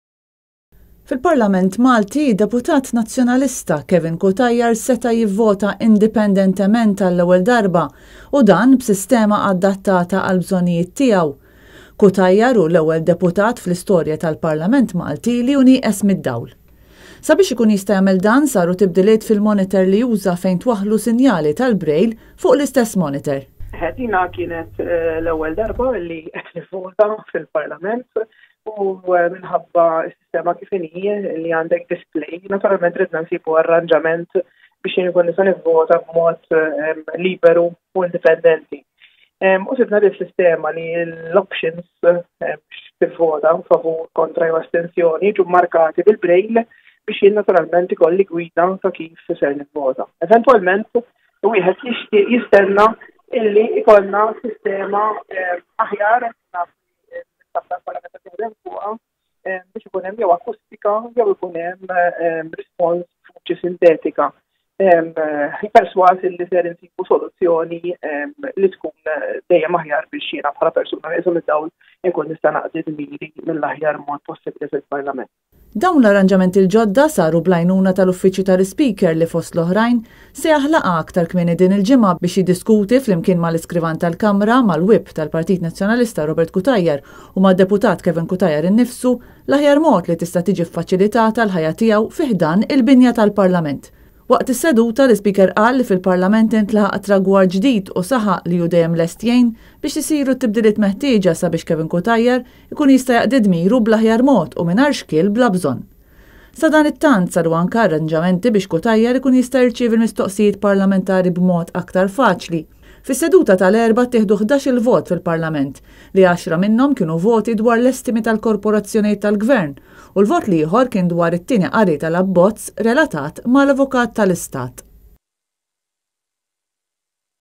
L-ewwel vot bil-Braille fil-Parlament Malti
Ilbieraħ fil-Parlament Malti id-Deputat Nazzjonalista Kevin Cutajar seta’ jivvota indipendentement għall-ewwel darba u dan b’sistema addattata għall-bżonnijiet tiegħu.  Cutajar hu l-ewwel Deputat fl-istorja tal-Parlament Malti li hu nieqes mid-dawl.